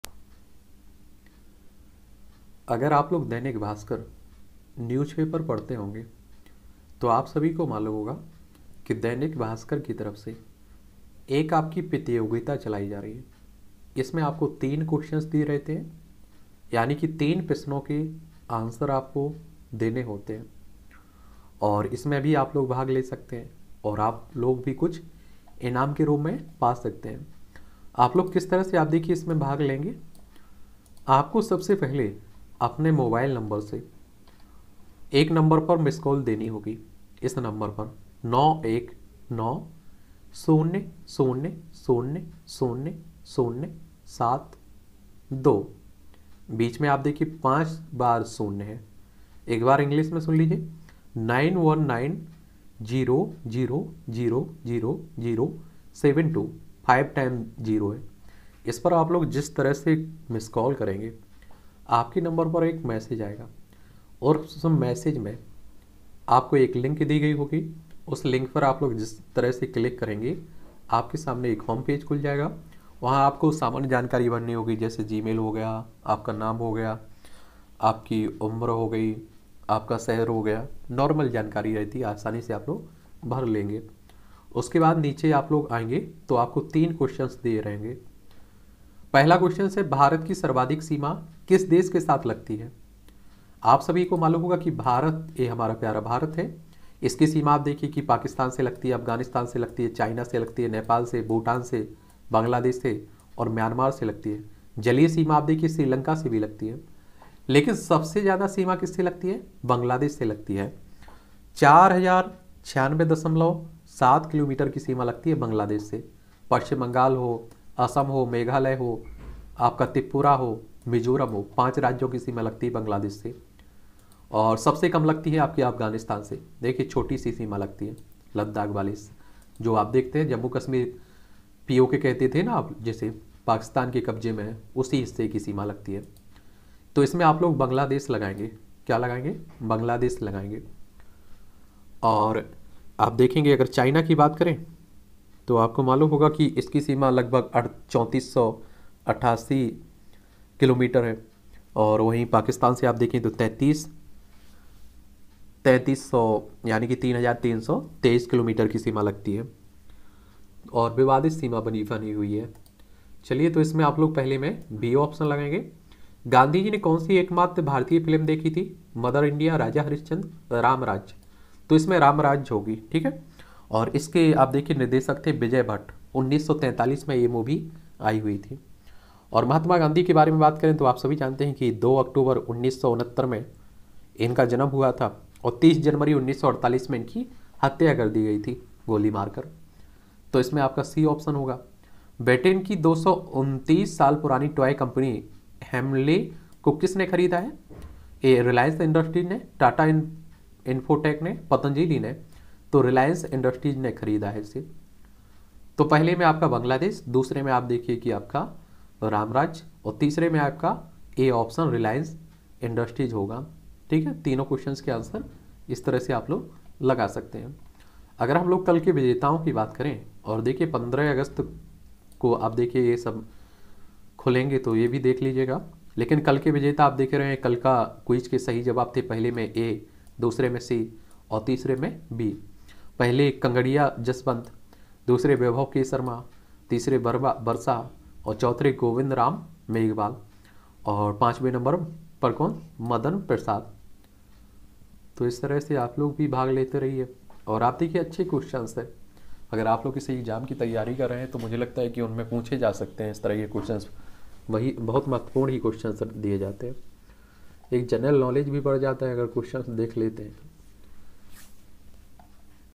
Aao Jane Apna Bharat Quiz for 14 August 2022 || दैनिक भास्‍कर क्विज || Dainik Bhaskar Quiz
यह ‘आओ जानें अपना भारत’ क्विज (Aao Jane Apna Bharat Quiz for 14 August 2022) कॉन्टेस्ट दैनिक भास्कर के पाठकों के लिए हैं। भास्कर के कर्मचारी, परिवार और एजेंट इस कॉन्टेस्ट में भाग नहीं ले सकेंगे। प्रतियोगिता से संबंधित सभी अधिकार दैनिक भास्कर के पास सुरक्षित हैं। वाद-विवाद का हल भोपाल न्यायालय के अधीन होगा।  क्विज का यह ऑनलाइन लिंक सुबह 6.00 बजे से रात 8 बजे तक खुला रहेगा। आजादी के 75 साल के उत्सव के तहत ‘आओ जानें अपना भारत’ क्विज में हिस्सा लेकर नॉलेज बढ़ाएं। साथ ही 14 अगस्त तक रोज 11000, 5100, 3100, 2100 और 1100 रु मूल्य के 5 इनाम जीतनेे का मौका। सही जवाब देने वालों में से लाटरी पद्धति से रोज 5 विजेता चुने जाएंगे। यही नहीं, सही जवाब देने वाले सभी प्रतिभागी 15 अगस्त के बंपर ड्रा में भी शामिल होंगे। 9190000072 पर मिस्ड कॉल देकर क्विज में आप आसानी से शामिल हो सकते हैं .

1. भारत की सर्वाधिक सीमा किस देश के साथ लगती है?
A. चीन B. बांग्लादेश C नेपाल
2. गांधीजी ने कौन सी एकमात्र भारतीय फिल्म देखी थी?
A. मदर इण्डिया B. राजा हरिश्चंद्र C. रामराज्य
3. ब्रिटेन की 229 साल पुरानी टॉय कंपनी हेमले को किसने खरीदा है?
A. रिलायंस इंडस्ट्रीज B. टाटा इन्फोटेक C. पतंजलि